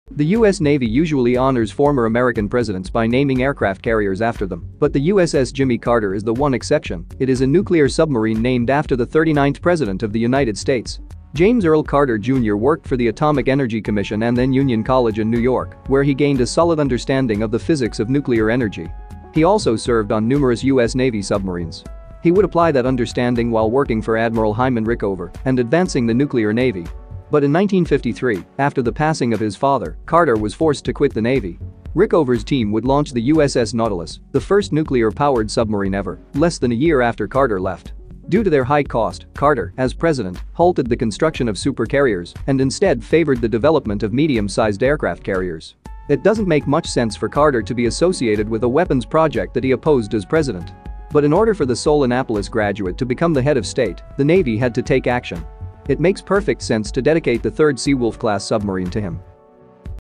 Why This Submarine Isn't An Aircraft Carrier?
The US Navy usually honors former American Presidents by naming aircraft carriers after them, but the USS Jimmy Carter is the one exception, it is a nuclear submarine named after the 39th President of the United States! James Earl Carter junior worked for the Atomic Energy Commission and then Union College in New York, where he gained a solid understanding of the physics of nuclear energy. He also served on numerous US Navy submarines. He would apply that understanding while working for Admiral Hyman Rickover and advancing the nuclear Navy. But in 1953, after the passing of his father, Carter was forced to quit the Navy. Rickover's team would launch the USS Nautilus, the first nuclear-powered submarine ever, less than a year after Carter left. Due to their high cost, Carter, as President, halted the construction of supercarriers and instead favored the development of medium-sized aircraft carriers. It doesn't make much sense for Carter to be associated with a weapons project that he opposed as president. But in order for the sole Annapolis graduate to become the head of state, the Navy had to take action. It makes perfect sense to dedicate the third Seawolf-class submarine to him.